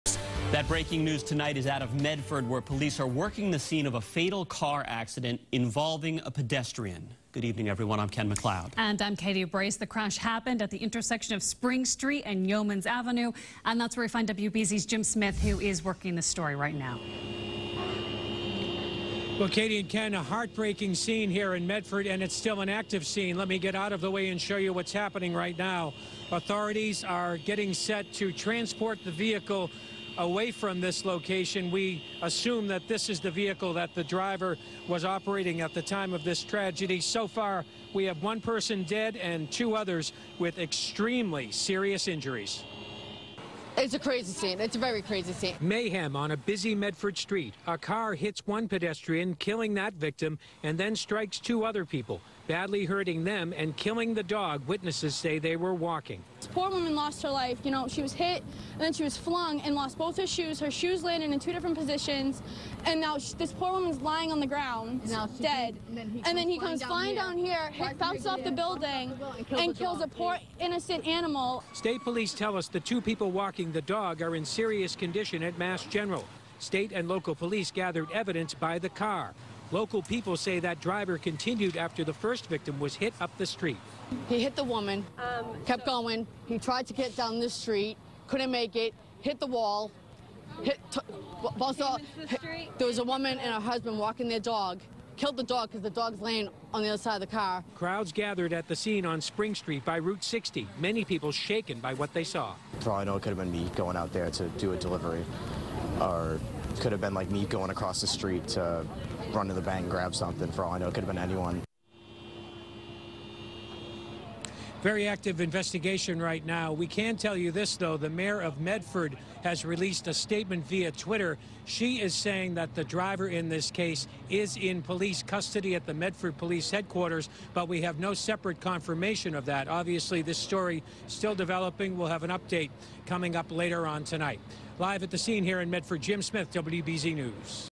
Driver In Custody After 1 Killed, 2 Injured In Medford Pedestrian Crash
A young driver is in custody after one person was killed and two others were seriously injured when they were hit by a car in Medford on Sunday. WBZ-TV's Jim Smith reports.